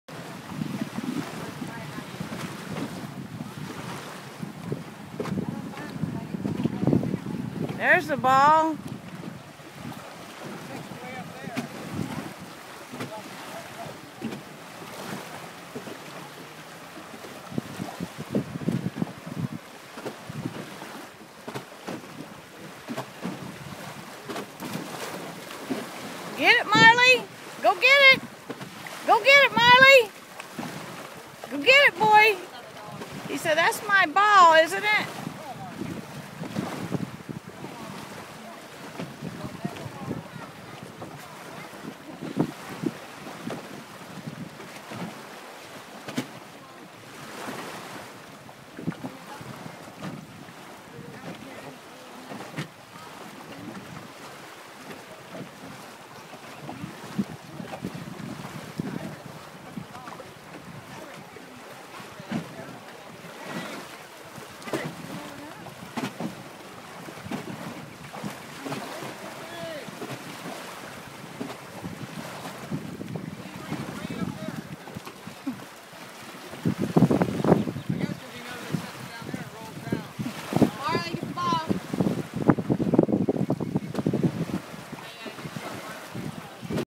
Marlee the puli and Rebel the Pit Bull swimming in the Gulf of Mexico
Marley  the Puli and Rebel the pit bull swimming in the summer month in the gulf of Mexico in Crystal River Florida.  Always beautiful weather in Florida